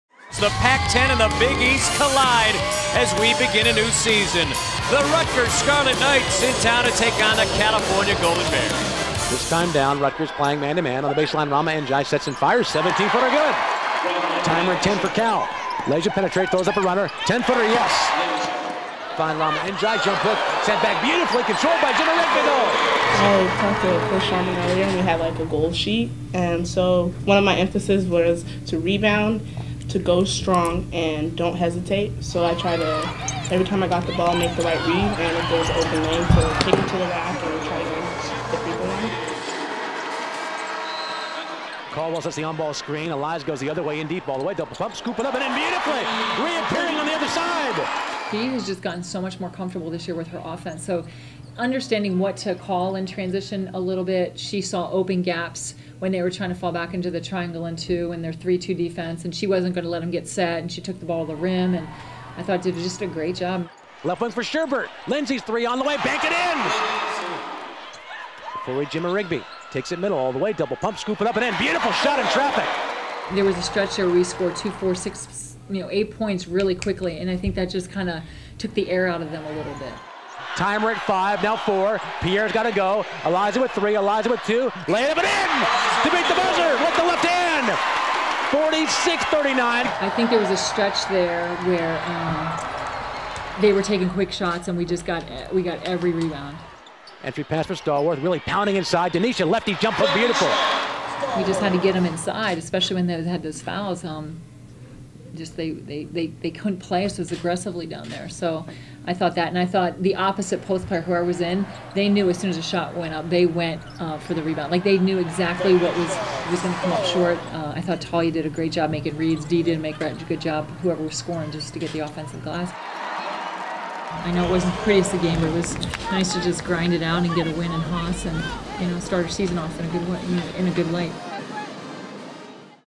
Highlights: Cal vs. Rutgers
The Golden Bears notched their first victory of the season, downing Rutgers 66-57. Layshia Clarendon paced the Bears with 16 points, while DeNesha Stallworth contributed a double-double (12 points, 10 rebounds).